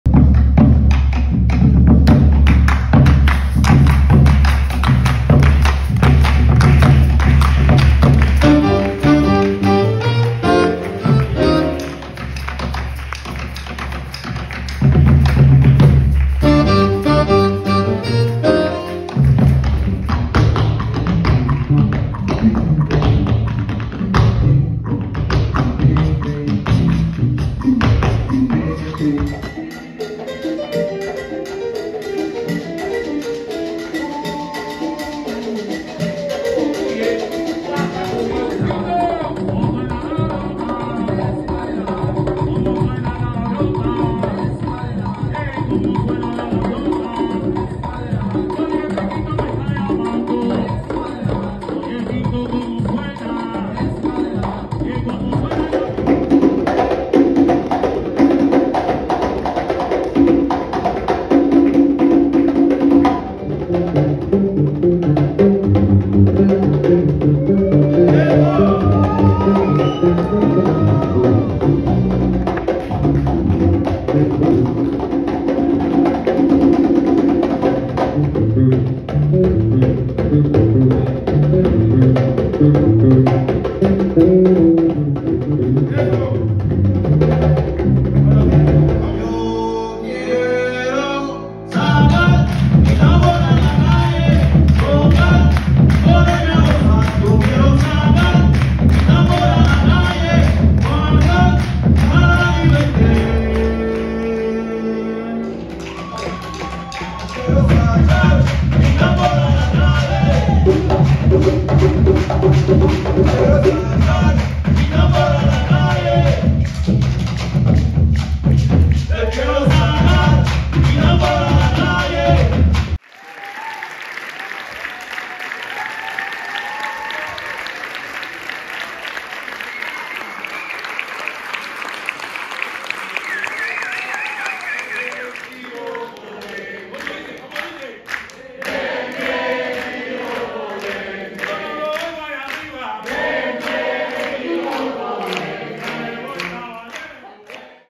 Concert Tambor Y Canto, rythmes et chants d’Amérique latine - 9 décembre 2023
Quatre maestros percussionnistes chantent, dansent, déclament, frappent leurs tambours, et vous font partager la richesse des chants et des rythmes ancestraux de leurs traditions musicales et dansées.

La fusion opère grâce à la compagnie enthousiaste du piano, du saxophone et de la contrebasse, qui rivalisent de virtuosité pour basculer d'un pays à l'autre et finalement aboutir à une musique inédite, inclassable, entre jazz et musiques traditionnelles. Un univers envoûtant fait de rythmes d'origine africaine, de mélodies amérindiennes, de textes d'Europe ancienne, et de créations actuelles.

Avec :
Papito Angarica : voix, percussions cubaines
Patinho Axé : voix, percussions brésiliennes
Rocco Sedano : danses, percussions d'Argentine
Walter "El Chato" Rebatta : voix, percussions colombiennes
Olivier Temime : saxophone
Willy Quiko : contrebasse
Simon Bolzinger : piano, direction, composition